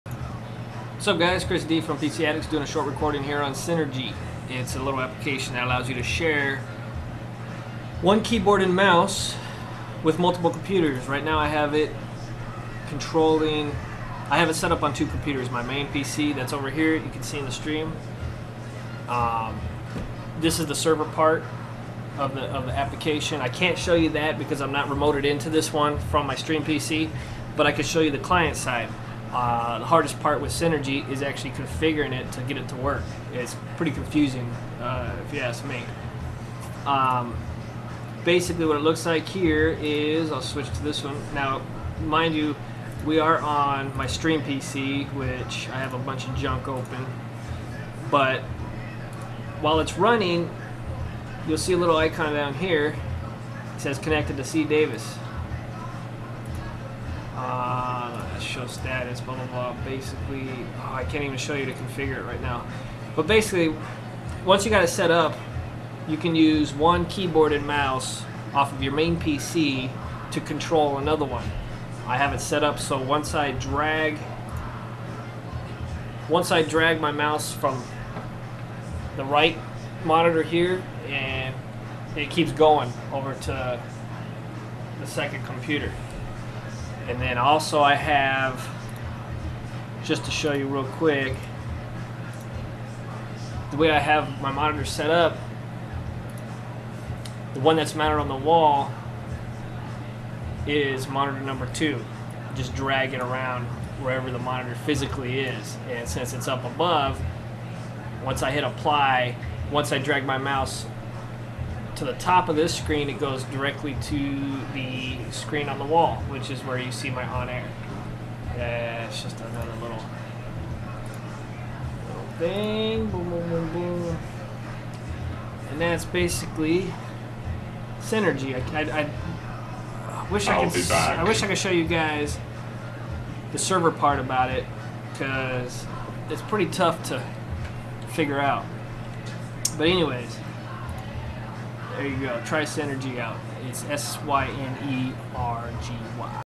Synergy - Share 1 Keyboard and Mouse Using Software
Share your keyboard and mouse with multiple computers.